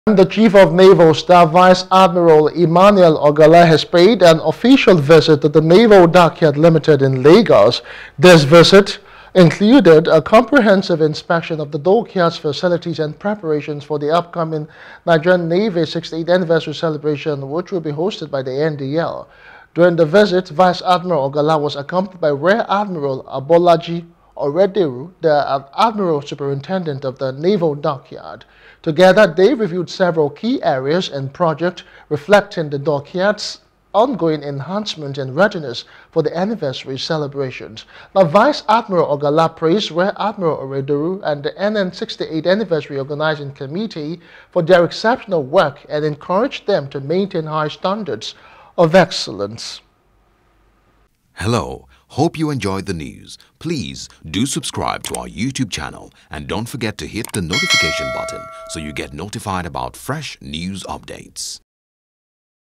68th Anniversary: Chief Of Naval Staff Inspects, Commissions New Project At Dockyard
The Chief of the Naval Staff (CNS), Vice Admiral Emmanuel Ikechukwu Ogalla has paid an official visit to the Naval Dockyard Limited (NDL) in Lagos. This visit included a comprehensive inspection of the dockyard’s facilities and preparations for the upcoming Nigerian Navy (NN) 68th Anniversary Celebration, which will be hosted by NDL. During his visit, Vice Admiral Ogalla was accompanied by Rear Admiral Abolaji Orederu, the Admiral Superintendent of the Naval Dockyard (ASND).
Together, they reviewed several key areas and projects, reflecting the dockyard’s ongoing enhancements and readiness for the anniversary celebrations. Vice Admiral Ogalla praised Rear Admiral Orederu and the NN 68th Anniversary Organizing Committee for their exceptional work and encouraged them to maintain high standards of excellence.